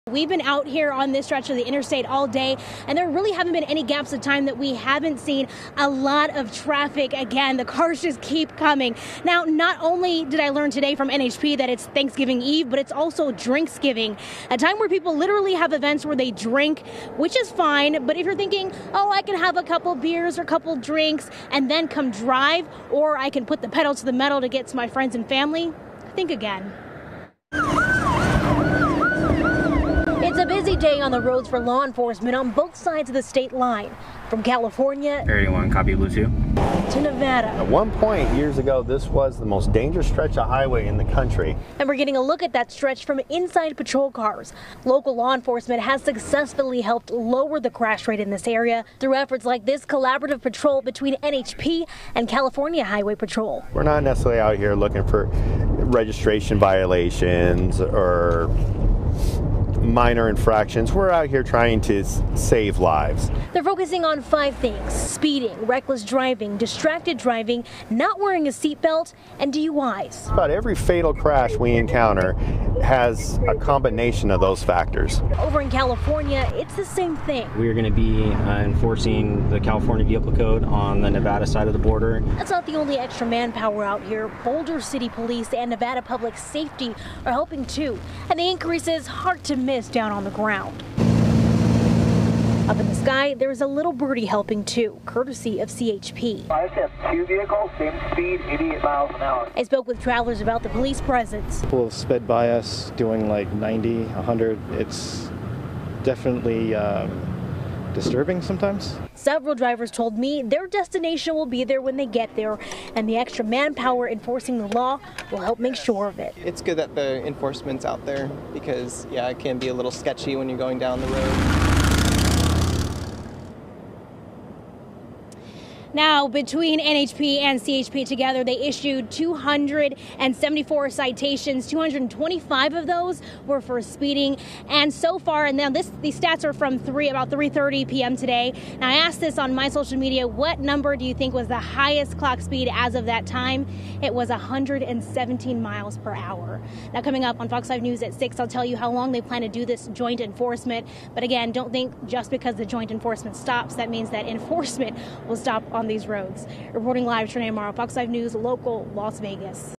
Extra troopers on the road for the holiday weekend
Extra NHP troopers will be on the road for the holiday weekend to crack down on drunk driving.